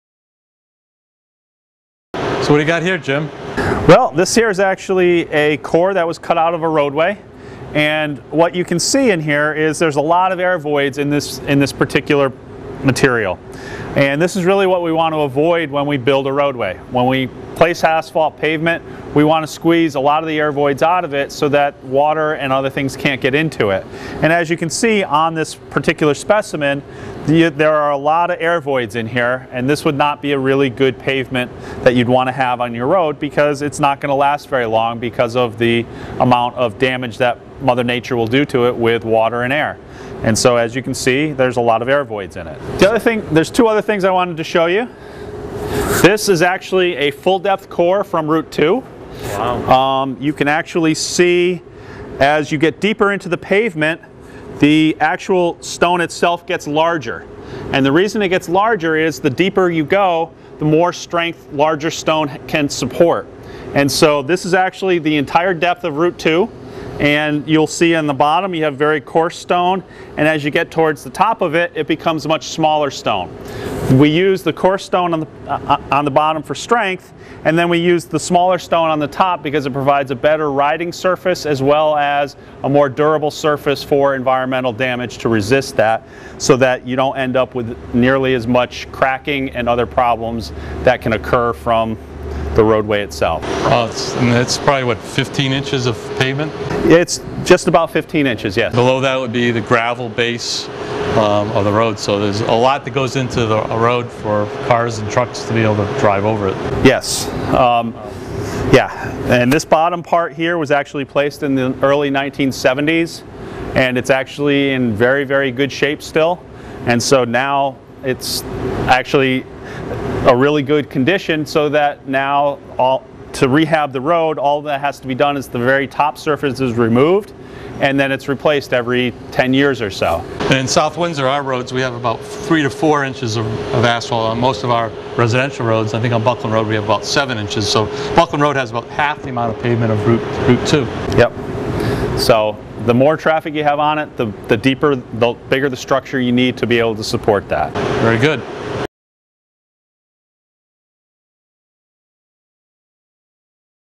Good Pavement vs Bad Pavement
Director of the Connecticut Transportation Institute James Mahoney uses two different road samples to demonstrate the difference between a good road (one that holds up against time and the elements) and a bad road.